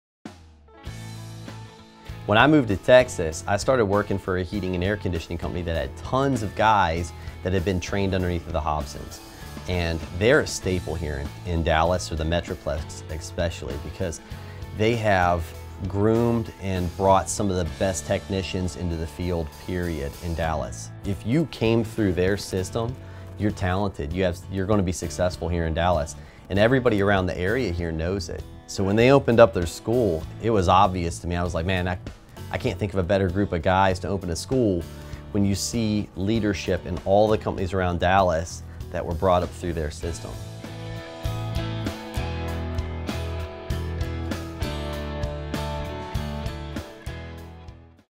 Perfect Technician Academy - A Staple in the DFW Metroplex
What an awesome testimonial from Rescue Air Heating and Cooling! We are so proud to be providing ready-to-work applicants for HVAC companies like this one.